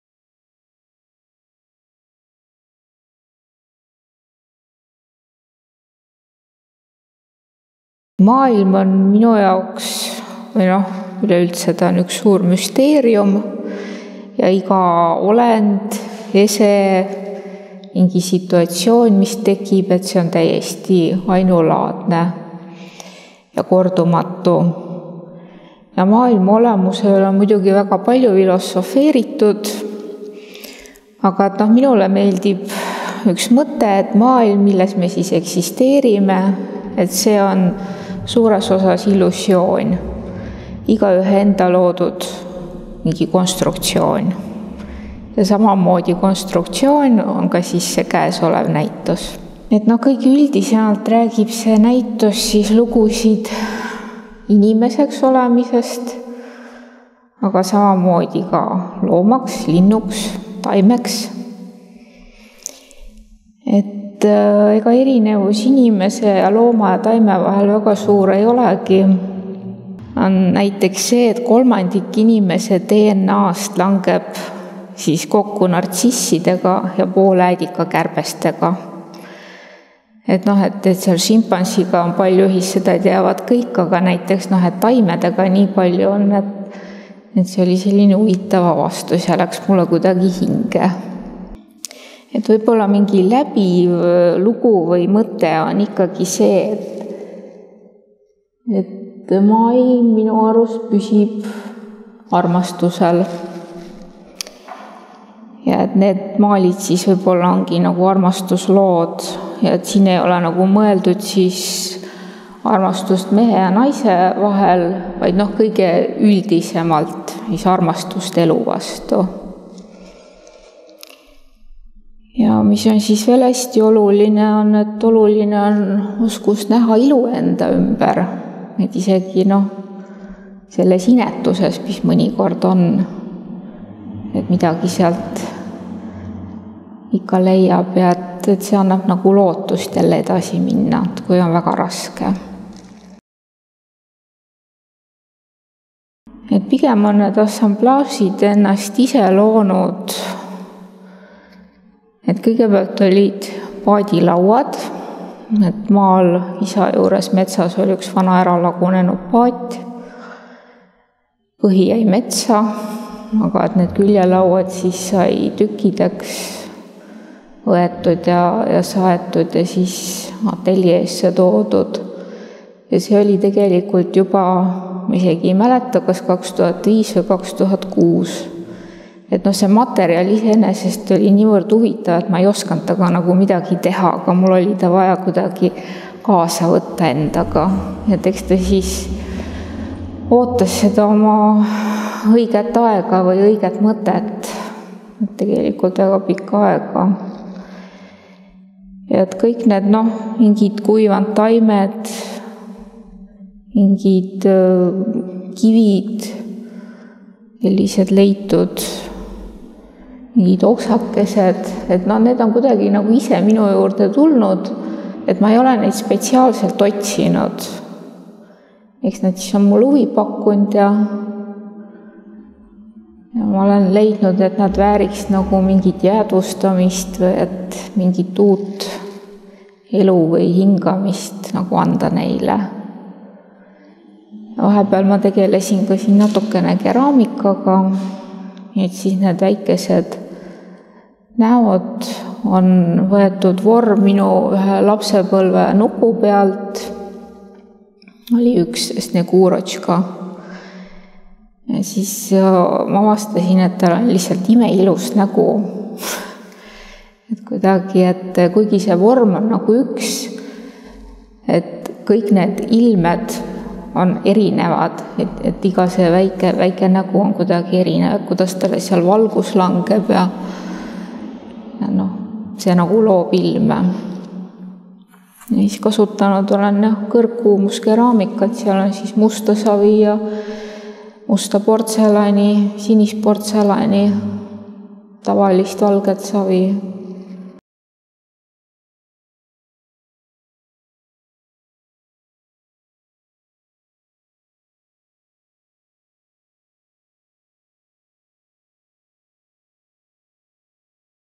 Meiu Münt "Maailma loomine"
Meiu Münt räägib oma Tartu Kunstimaja väikese saali näitusest "Maailma loomine" 11.04.–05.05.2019.

Näitusel olevad õlimaalide ja assamblaažid jutustavad lugusid inimeseks, taimeks või loomaks-linnuks olemisest. Lisaks tavapärasele lõuendile on kasutatud tõrvatud paadilaudu, kõrgkuumuskeraamikas valminud pisiplastikat, kerisekive, kuivatatud taimi ja teisi looduslikke materjale. Teosed on pigem pööratud sissepoole ning näivad esmapilgul vaikivat, kuid ootavad siiski, et iga külastaja looks oma loo ja maailma.

Kunstnik lisab: „Kui sulle on antud võime näha liivateras maailma, võid end pidada õnnelikuks inimeseks. Maailm on müsteerium: iga olend, tekkiv situatsioon, asi ja ese ses ilmas on ainulaadne. Maailm, milles eksisteerime, on suuresti illusioon, igaühe enda loodud konstruktsioon.”

Meiu Münt (snd 1972) on lõpetanud Tartu Ülikooli maaliosakonna. Ta osaleb näitustel alates 1997. aastast. 2014. aastal omistati talle Ado Vabbe preemia. Tema eelmised isikunäitused Tartu Kunstimajas toimusid aastatel 2011 ja 2014.

Meiu Münt talks about her exhibition "Creation of the World" in the small gallery of the Tartu Art House 11.04.–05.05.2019.

The oil paintings and assemblages at the exhibition talk about being people, plants, animals and birds. In addition to regular canvases, Münt has also used tarred boat planking, small sculptures in high temperature ceramics, stones from a sauna stove, dried plants and other natural materials. The works are turned inwards and, at first glance, seem to be silent, but still want the visitors to create their own stories and worlds.

The artist adds: “If you have been given the ability to see a world inside a grain of sand, you can consider yourself a fortunate person. The world is a mystery: every being, emerging situation, every object in this world is unique. The world in which we exists is largely an illusion, a construction created again by every person.”

Meiu Münt (b 1972) has graduated from the Painting Department of the University of Tartu. She has participated in exhibitions since 1997. In 2014, she was awarded the Ado Vabbe Art Prize. Her previous personal exhibitions at the Tartu Art House were in 2011 and 2014.